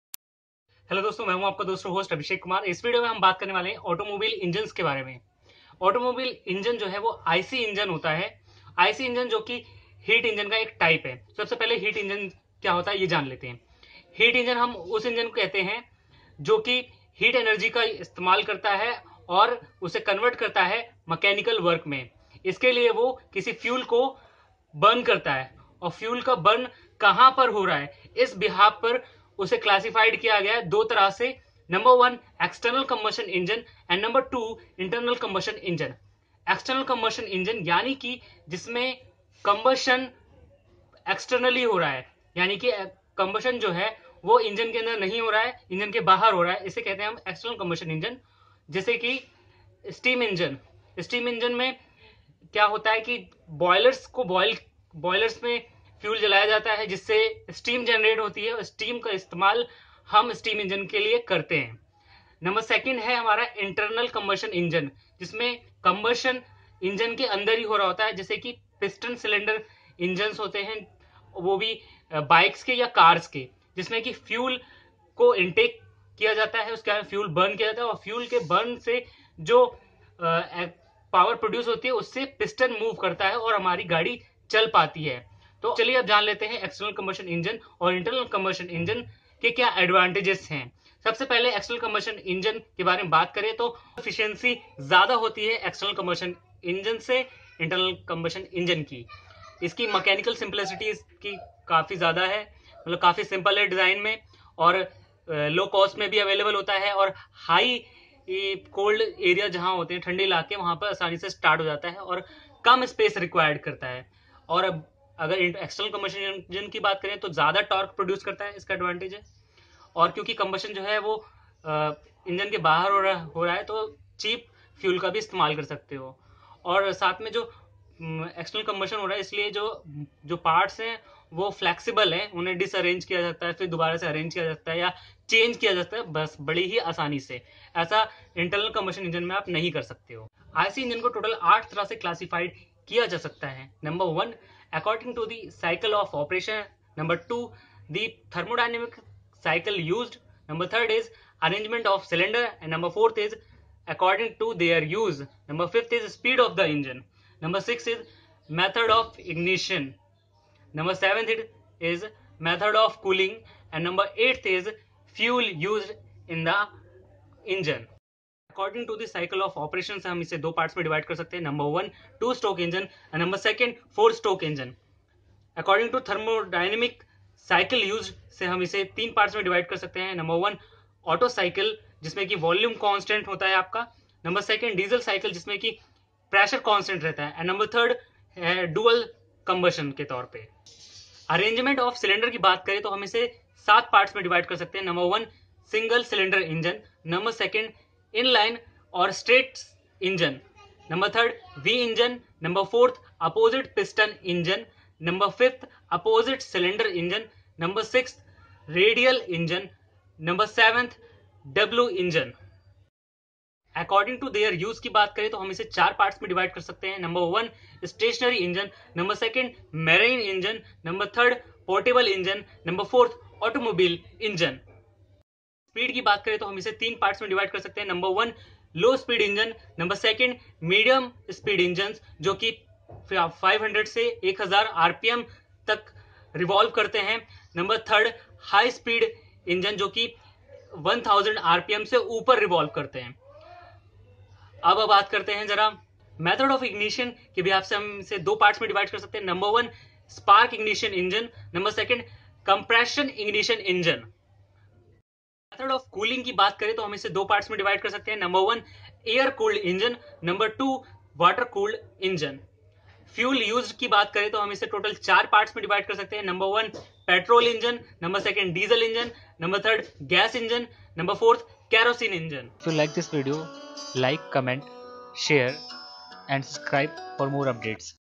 Classification of automobile engine || Heat engine classification || IC engine classification
generally ic engines are used as automobile engine so classification of external combustion engine is not in the video